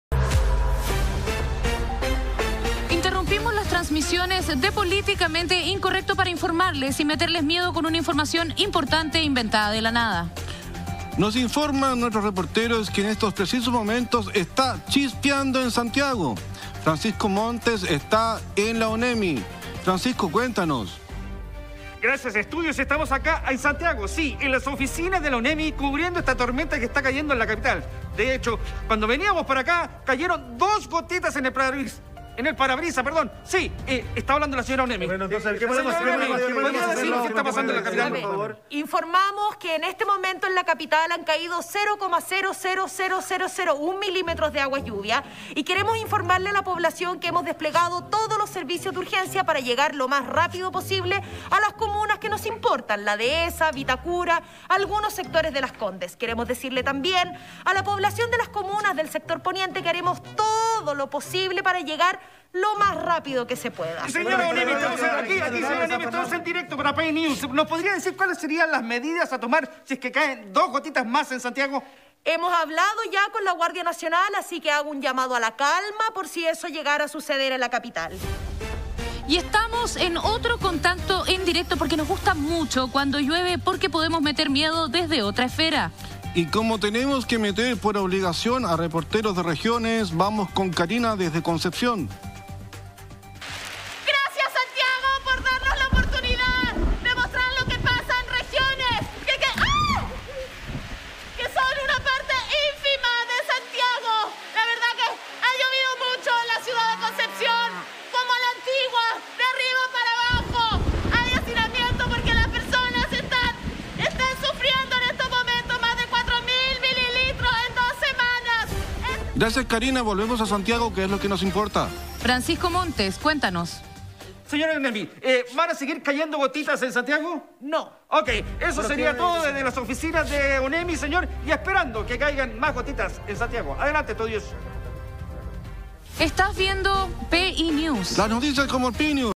Breaking Nius: ¡Caen gotas en Santiago!
Interrumpimos nuestra programación para informar de esta tormenta que afecta a la capital.